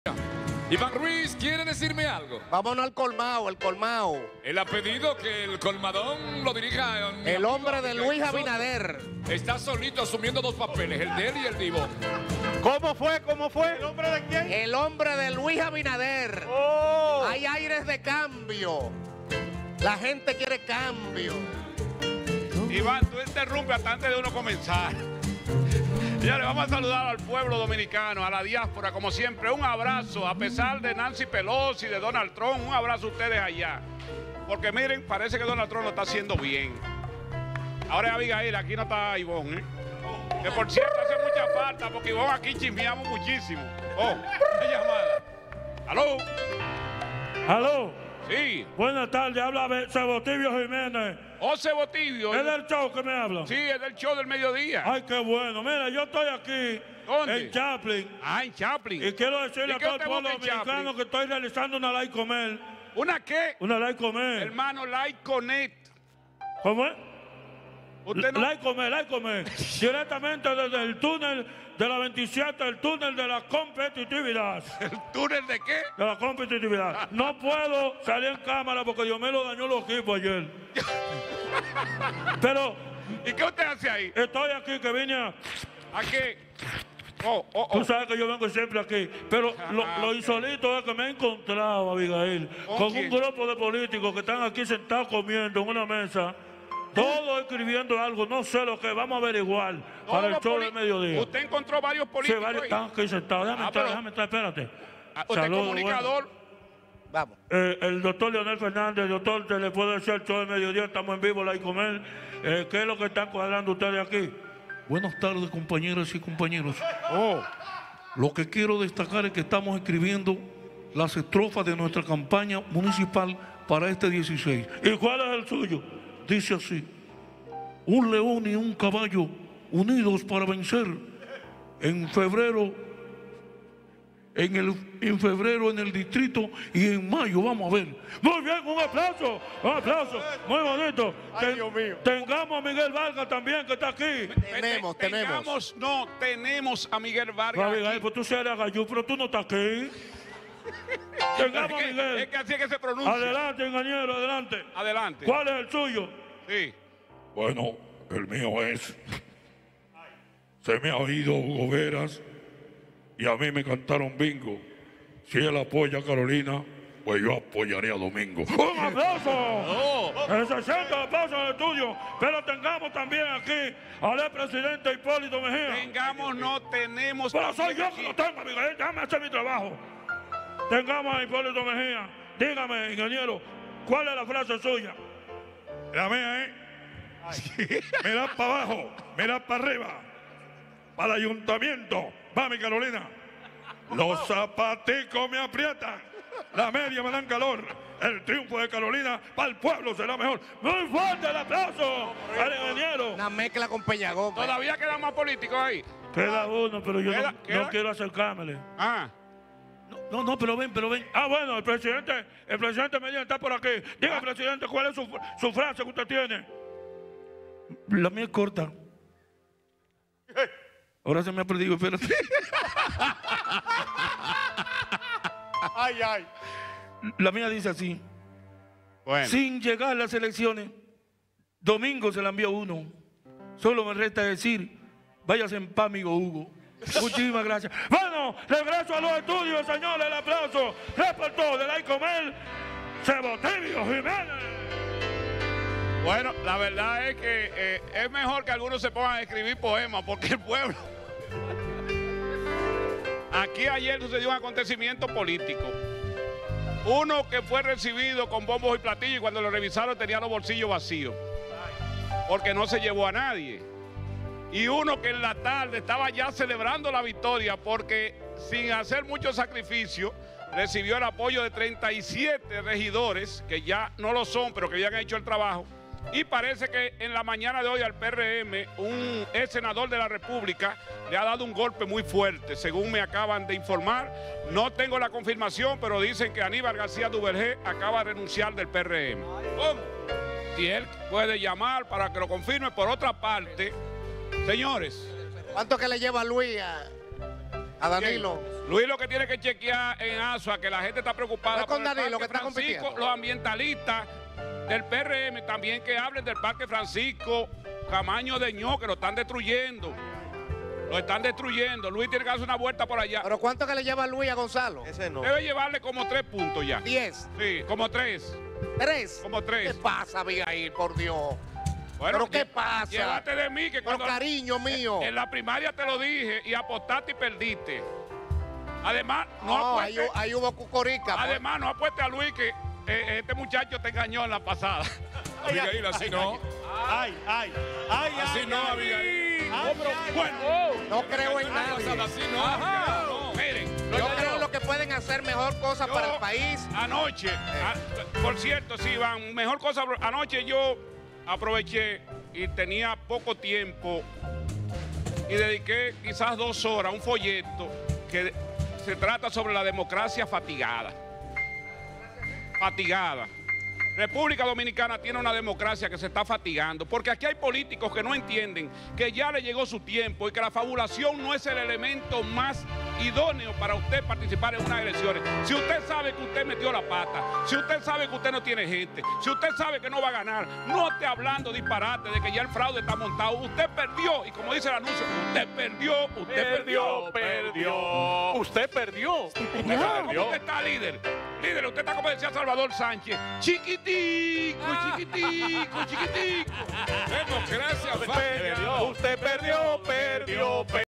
Colmado la Costumbre: Democracia fatigada
En el show del mediodía, segmento colmado la costumbre con Abigail Soto, este nos actualiza los temas más reciente de la política dominicana.